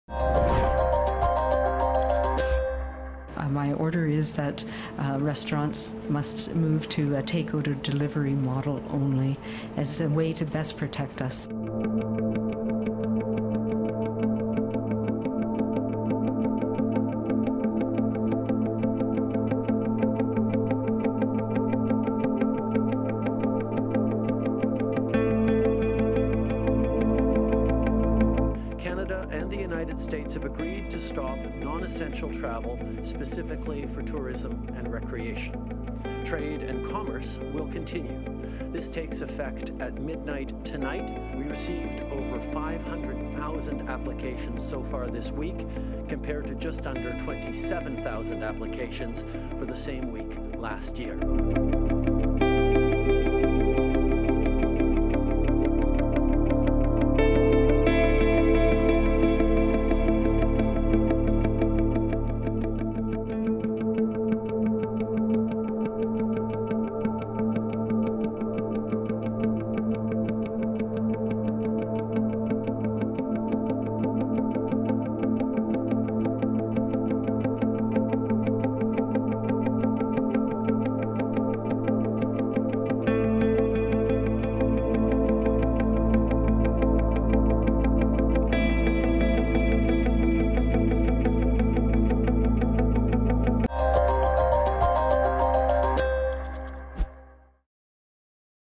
Here's What's Making News Mar. 20, 2020
From the province banning dine-in at restaurants to prevent the spread of COVID-19, to Italy seeing its worse day of deaths, here are some of the stories at citynews1130.com and live on-air at player.news1130.com